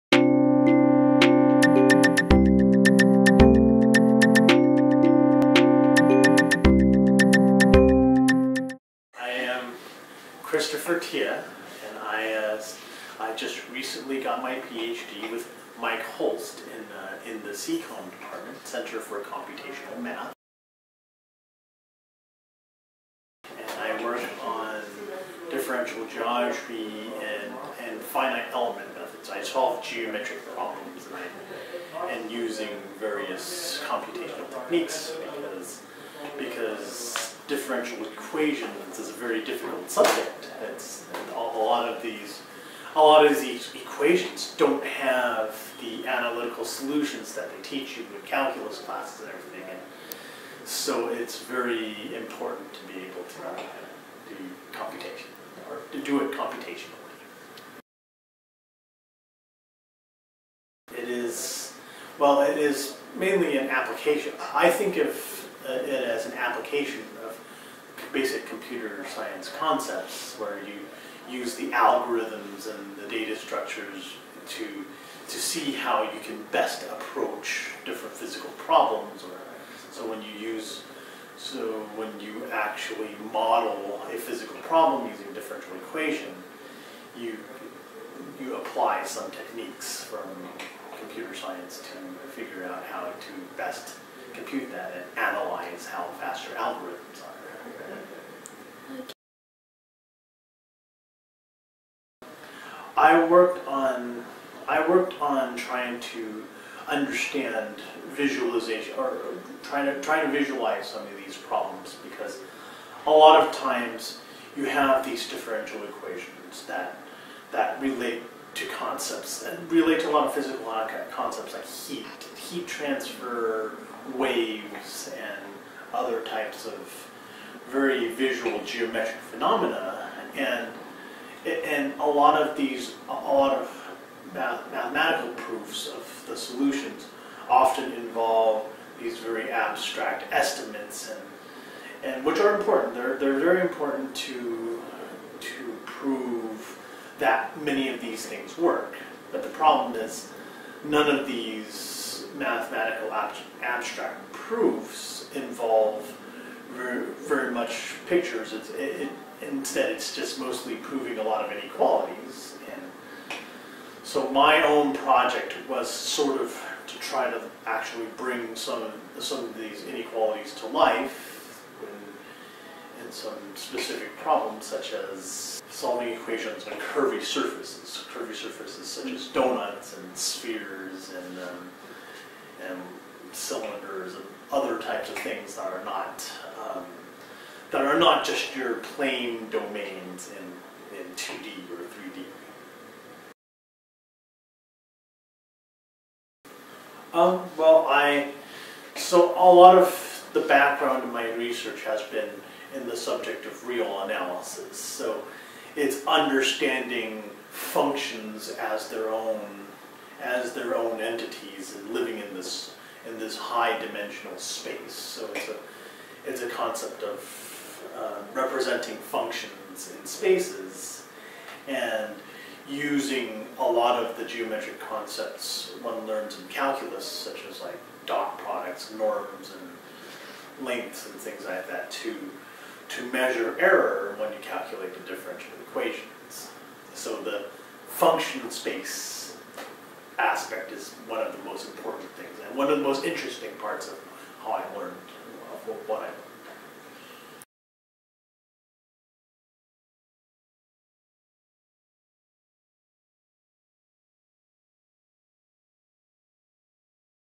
Meet Christopher Tiee, Researcher in Computational Mathematics
Dr. Tiee is a professor in the UCSD math department as well as a researcher at the Center for Computational Math. In this video, Dr. Tiee shared his passion in mathematical modeling and past research projects, illustrating ways of transforming 2D models in to applicable 3D models. His experience brings to light the diverse possibilities available to math majors and the opportunities to work at the cutting edge of mathematical reasoning here at UCSD.

0:00 - Intro
0:20 - Can you tell us about your research?
0:59 - How is Computational Math different from Computer Science
1:40 - Can you tell us about a project you’ve worked on?
3:15 - What is an important skill for computational math?